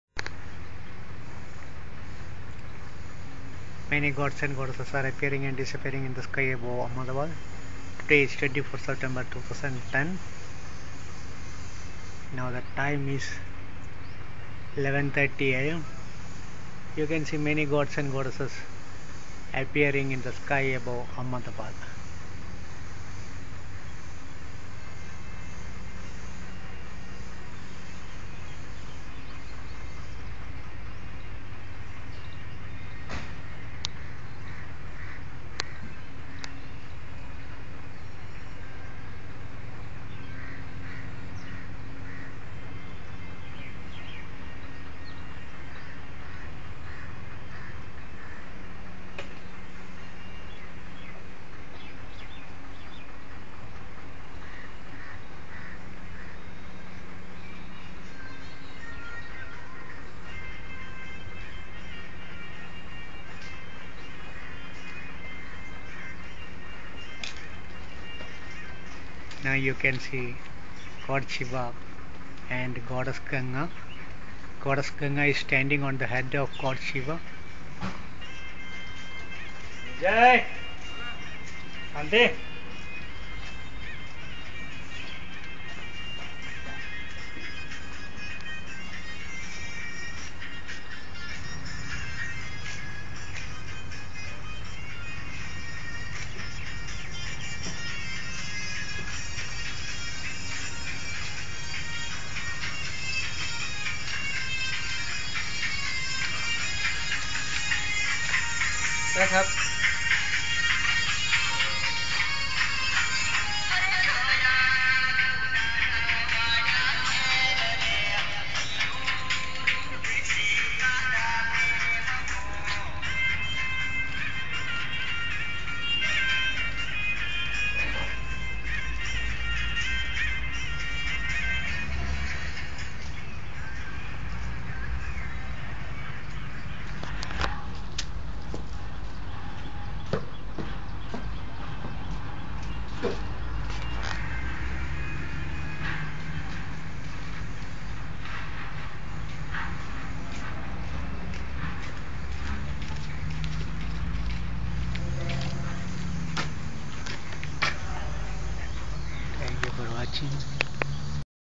NINE DURGA APPEARING IN SKY
See 9 Durga and lion appearing in sky.